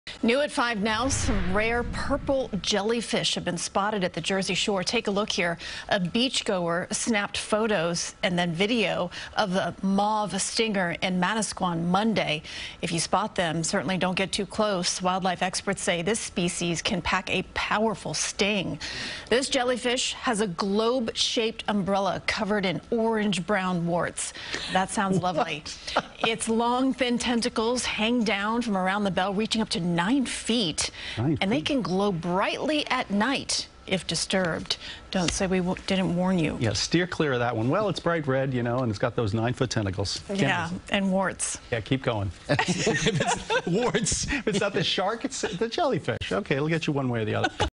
Rare mauve stinger jellyfish spotted along Jersey Shore
Some rare purple jellyfish have been spotted at the Jersey Shore.